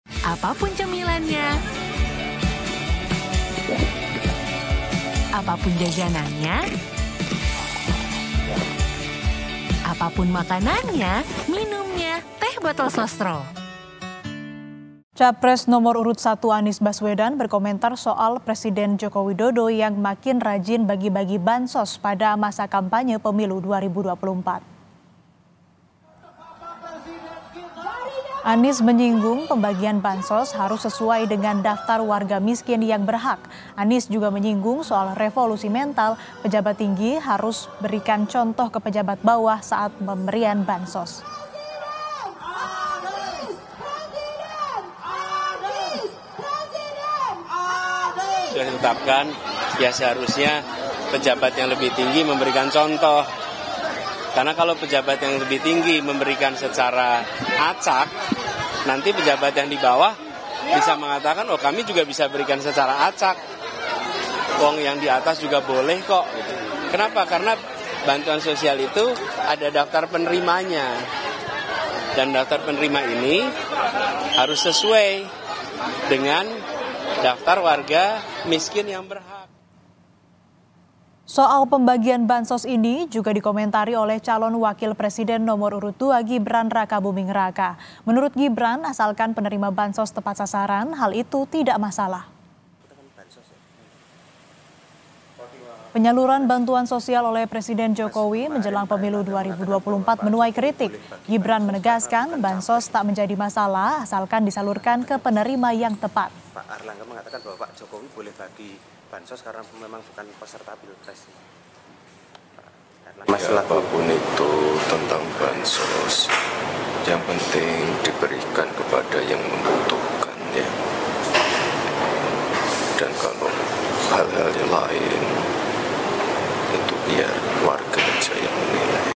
Beda Tanggapan Anies dan Gibran Soal Presiden Jokowi Bagi Bansos di Masa Kampanye Pemilu 2024
KOMPAS.TV - Capres Nomor Urut Satu, Anies Baswedan berkomentar soal Presiden Joko Widodo yang makin rajin bagi-bagi bantuan sosial pada masa Kampanye Pemilu 2024.

Anies menyinggung pembagian bansos harus sesuai dengan daftar warga miskin yang berhak. Anies juga menyinggung soal revolusi mental pejabat tinggi harus berikan contoh ke pejabat bawah saat pemberian bansos.

Soal pembagian bansos ini, juga dikomentari oleh Calon Wakil Presiden Nomor Urut 2, Gibran Rakabuming Raka. Menurut Gibran asalkan penerima bansos tepat sasaran hal itu tidak masalah.

Penyaluran bantuan sosial oleh Presiden Jokowi menjelang Pemilu 2024 menuai kritik. Gibran menegaskan bansos tak menjadi masalah asalkan disalurkan ke penerima yang tepat.

Baca Juga Sentil Pemerintah Lewat Pertanyaan Ganjar, Anies: Bansos Harus Dibagikan Melalui Jalur Birokrasi di

Satu Putaran Dua Putaran, Saksikan Hanya di KompasTV.